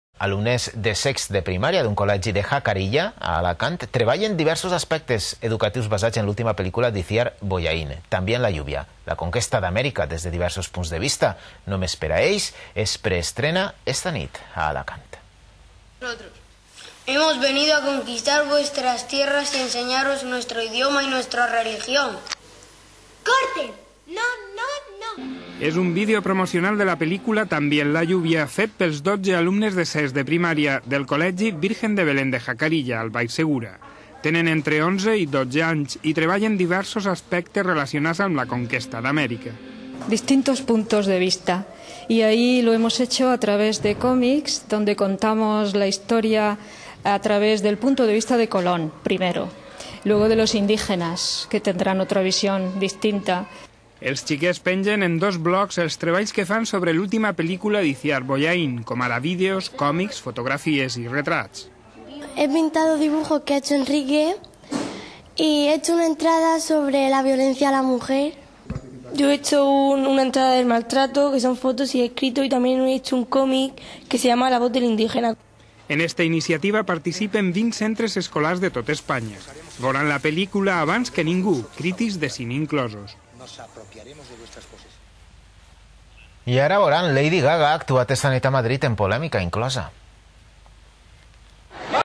Reportaje en la TV valenciana
Reportaje emitido el 13/12/2010 en la TV valenciana. Protagonistas: Blogmaníacos.